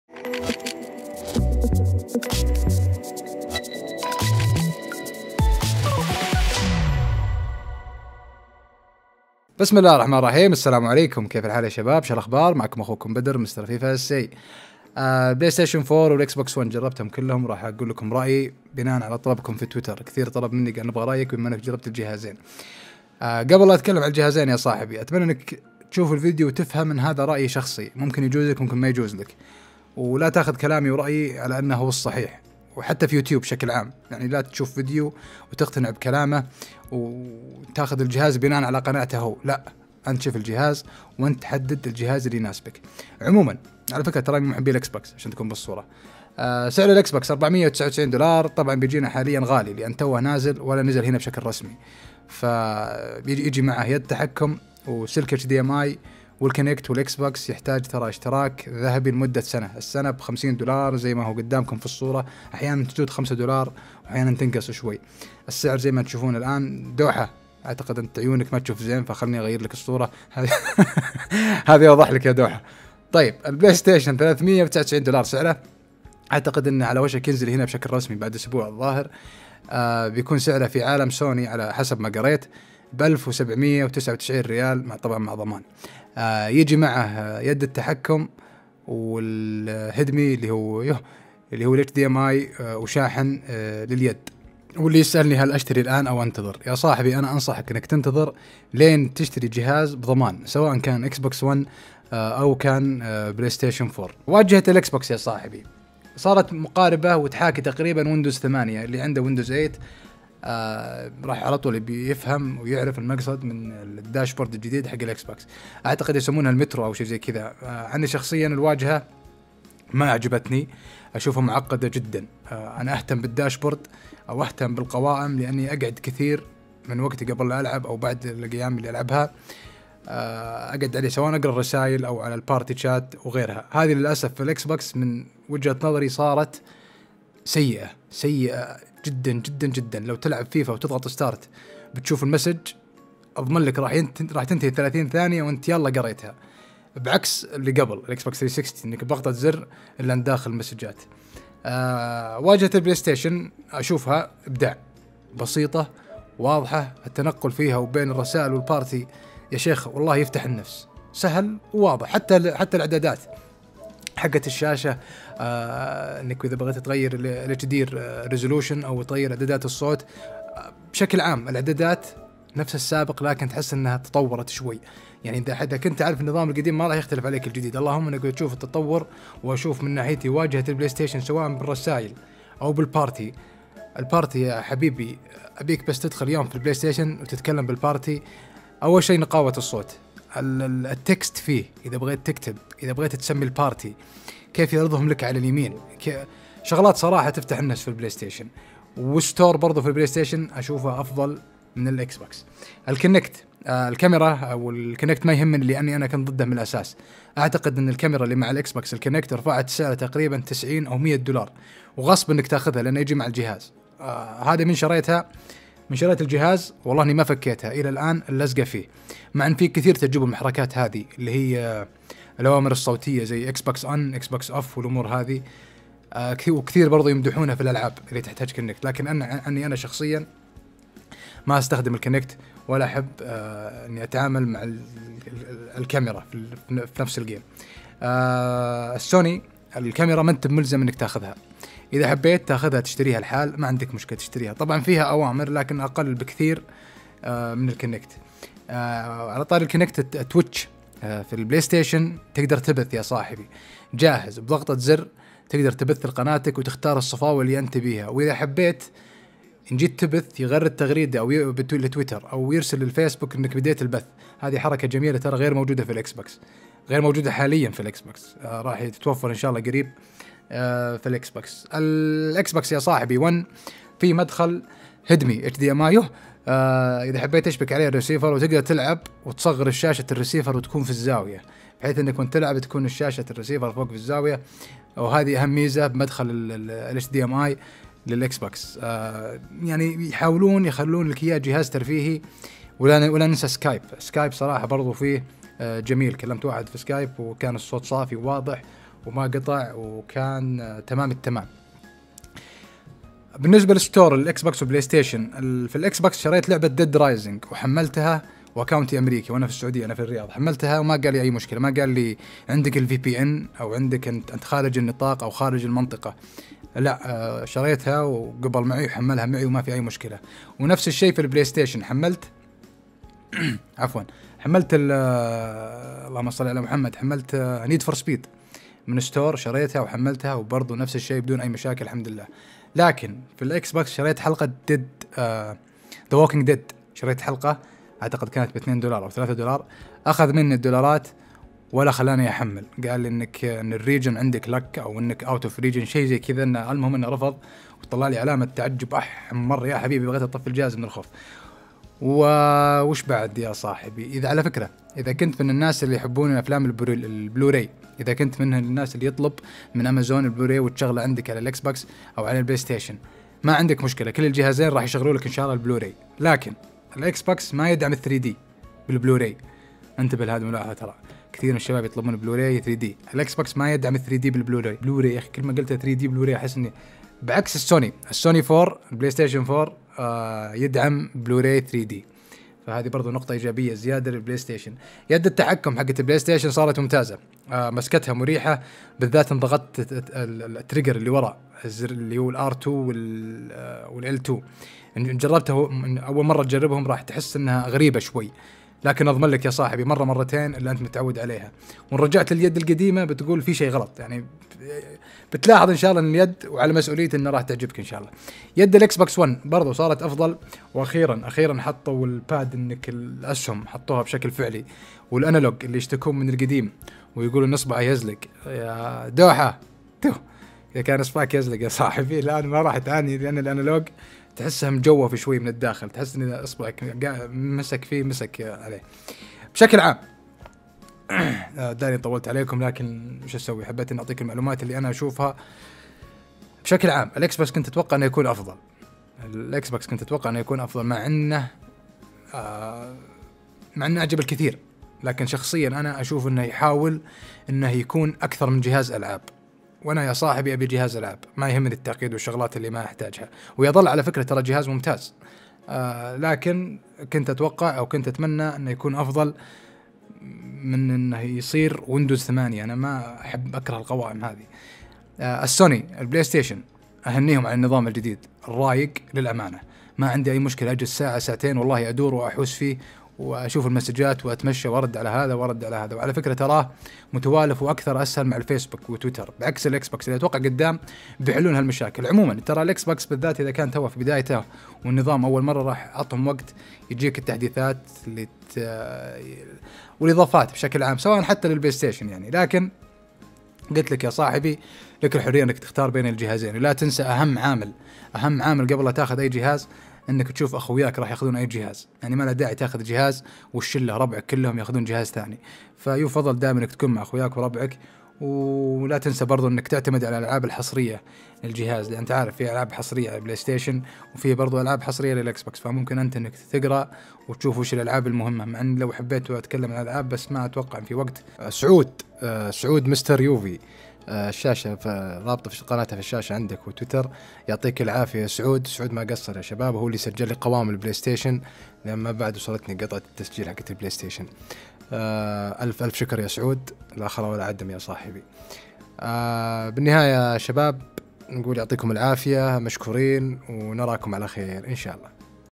البلاي ستيشن 4 والاكس بوكس 1 .. رأي شخصي
تم تعطيل التعليقات..النقاشات للاسف بالتعليقات غالبا ماتكون عقيمة وتصل للسب والشتم.
اكرر هذا الفيديو مجرد (رأي) واحد يلعب قيمز عادي حالي من حالك..فاالنهاية كل الاجهزة ممتازة

Skype: iMrFifaSA
XBOX360: MrFifaSA
PS3: MrFifaSA